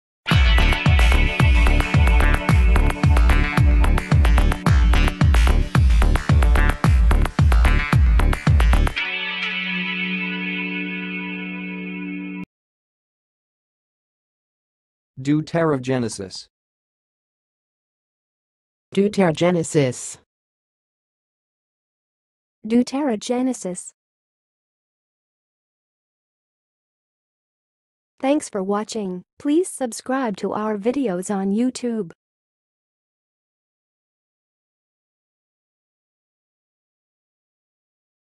How to say "deuterogenesis"! (High Quality Voices)
Watch in this video how to say and pronounce "deuterogenesis"!